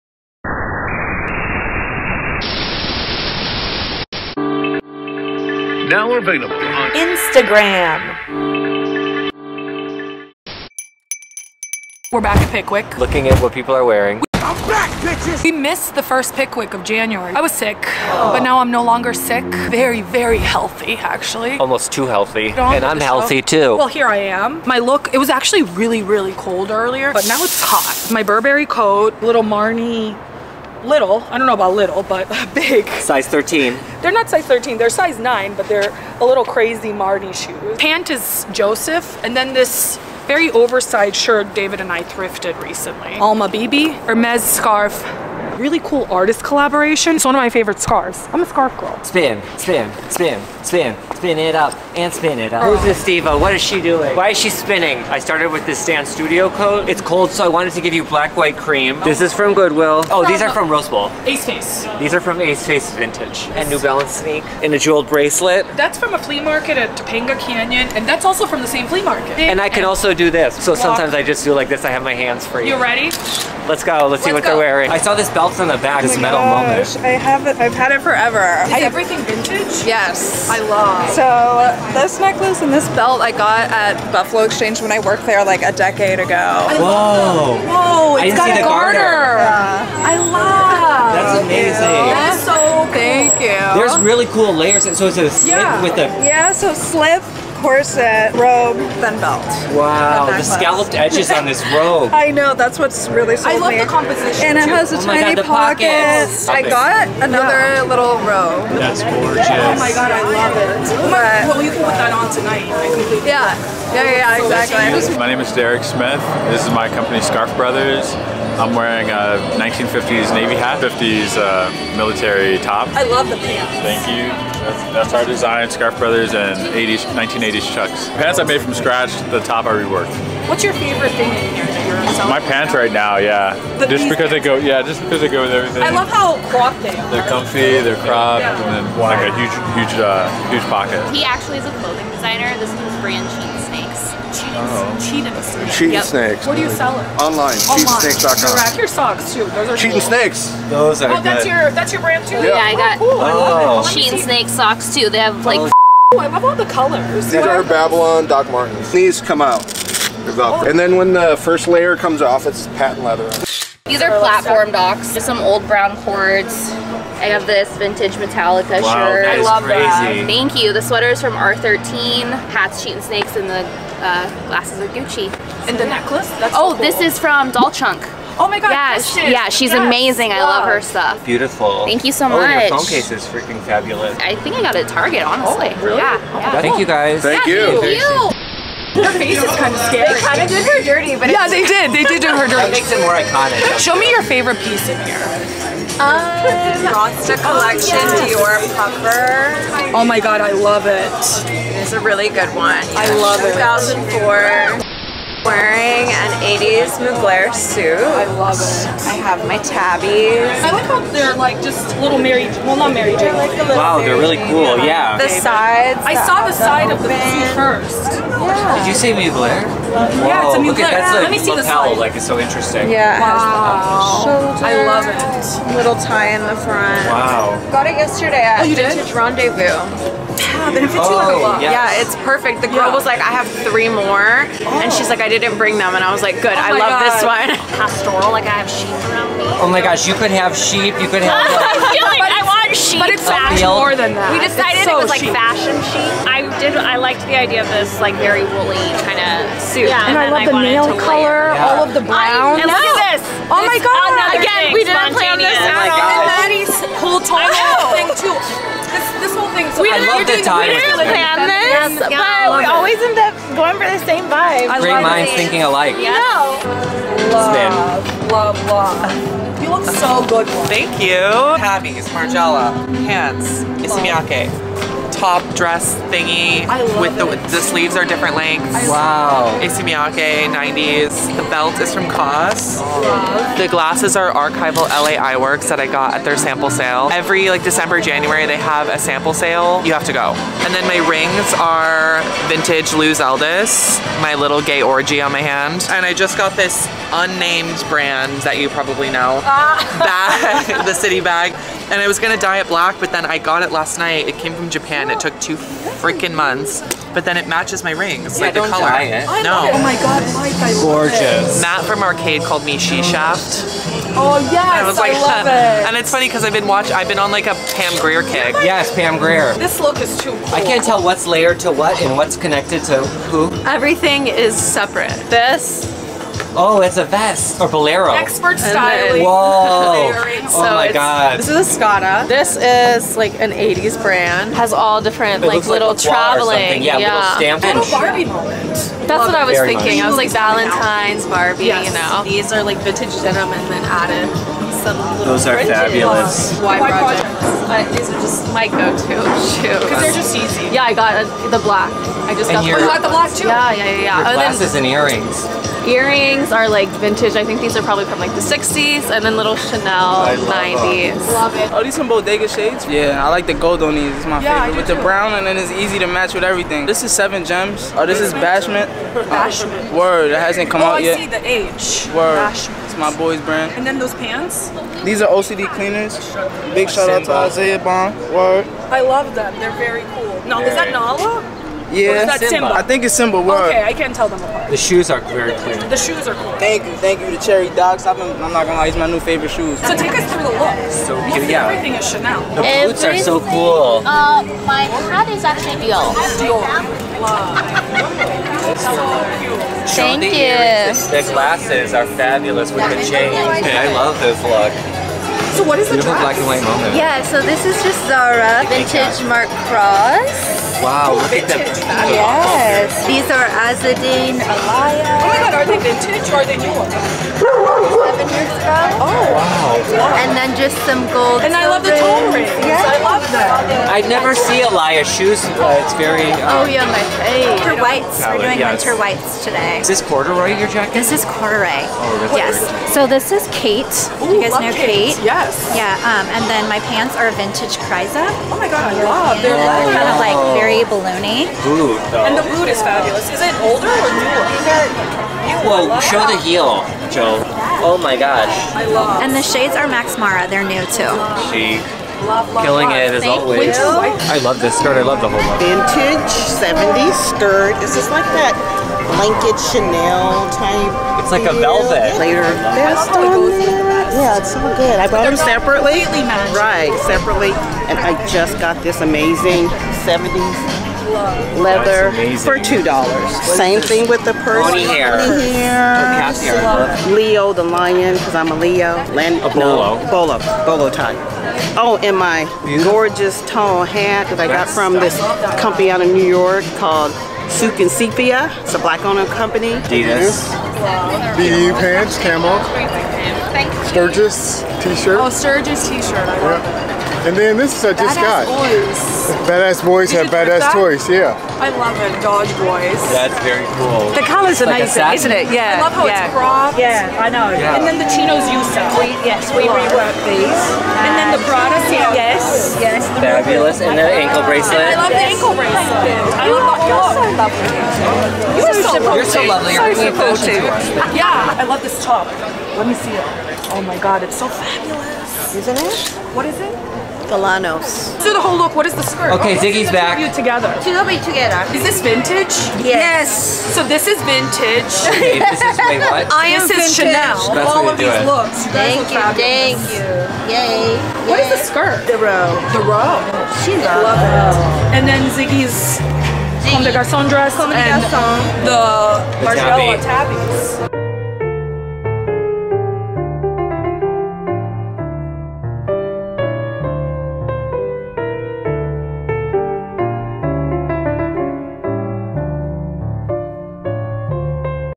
What Are People Wearing in L.A.? ft. PICKWICK VINTAGE SHOW (Los Angeles STREET STYLE) | EP.38
Hi Friends!
Welcome to another video of WHAT ARE PEOPLE WEARING IN L.A.? This is a special episode with so many fabulous looks at PICKWICK VINTAGE SHOW which was held at the ROW DTLA! This market happens only once a month (the next one is on APRIL 15). You can find more info re Pickwick Vintage on their or on their Instagram: @pickwickvintage ❤️
xoxo
Jasmina

Timecodes
0:00 - WELCOME
0:24 - OUR LOOKS
1:15 - LOOK 1
1:55 - LOOK 2
2:21 - LOOK 3, 4
3:25 - LOOK 5
3:58 - LOOK 6, 7
4:27 - LOOK 8
5:20 - LOOK 9
6:01 - LOOK 10
6:30 - LOOK 11
6:44 - LOOK 12
7:08 - LOOK 13
7:34 - LOOK 14, 15
8:24 - LOOK 16
8:41 - LOOK 17, 18